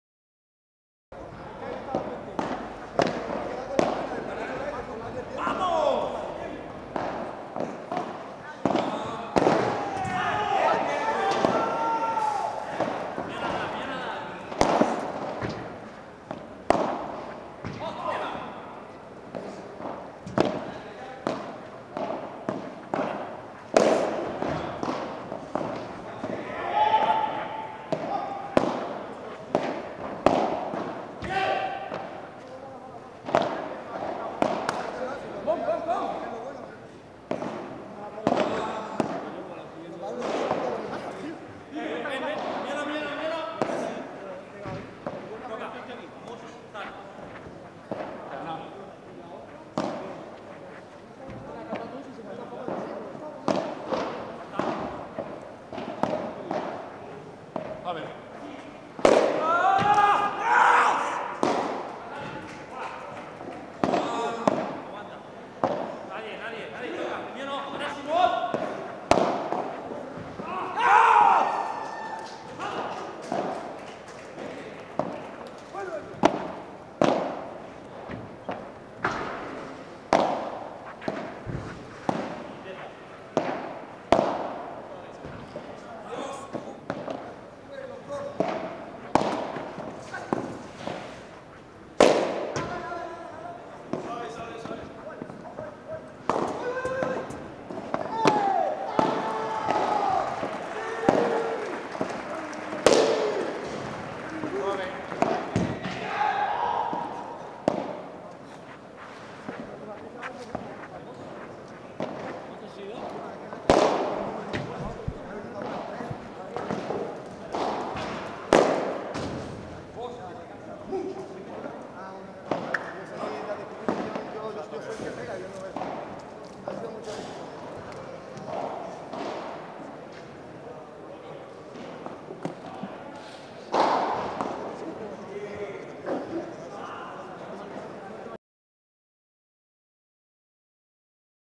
Vamos Pádel. David Alten y Christoph Krenn contra Mauricio Andrini y Gabriel Espelleta
Resumen del partido entre David Alten y Christoph Krenn contra Mauricio Andrini y Gabriel Espelleta en el WPT de Mijas 2018 en el Cerrado del Águila.
Resultado: 4-6 / 7-5 / 7-6